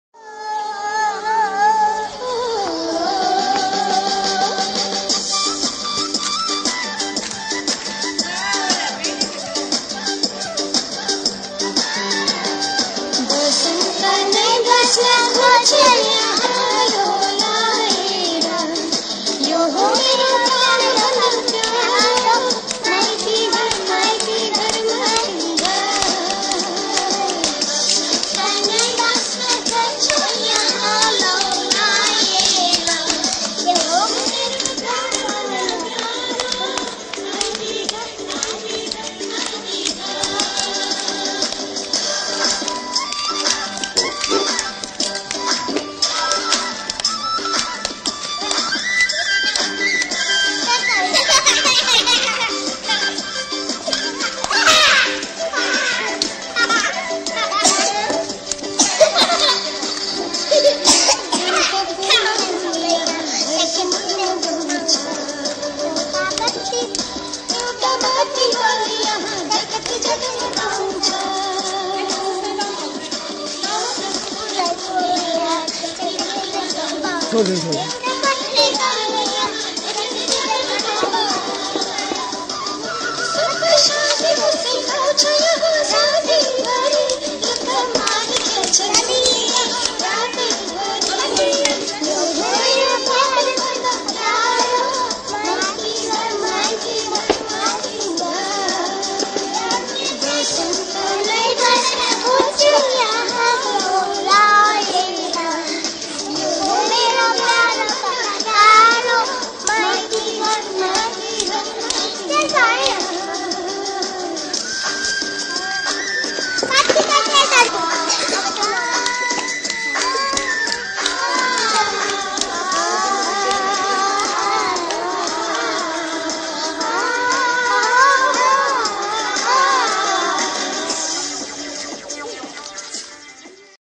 Kids Dancing In Nepali Songs(Toddlers Intl Pre School)
Kids dancing in free time.Just For fun.